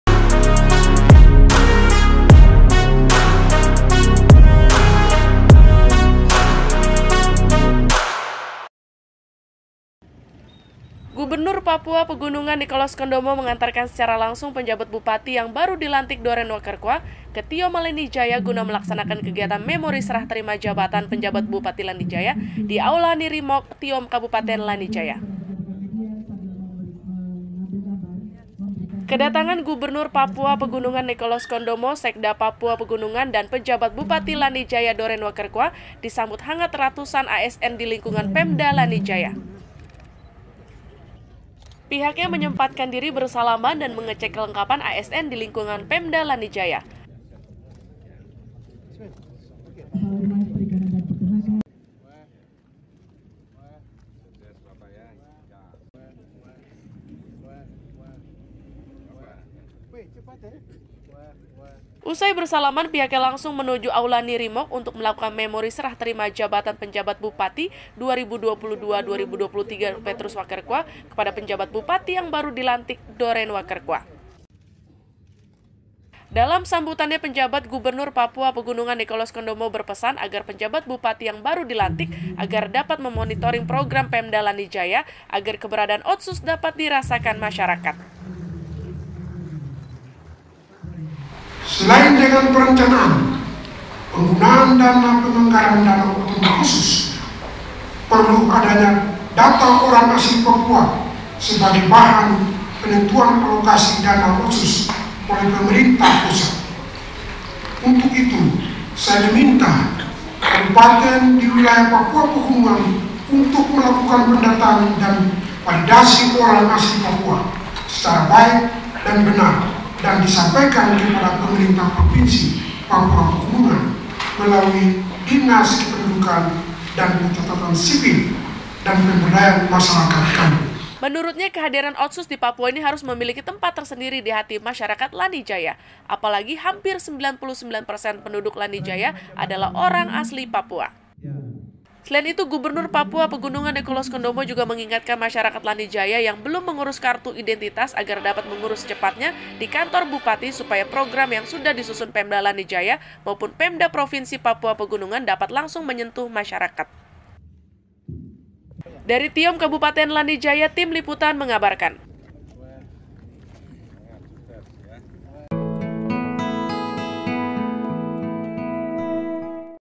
Memori Serah Terima Jabatan, Doren Wakerkwa diantar Pj Gubernur Papua Pegunungan
Serah Terima jabatan dari Pj Bupati Lanny Jaya periode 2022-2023 kepada Pj Bupati Lanny Jaya Doren Wakerkwa diantar Pj Gubernur Papua Pegunungan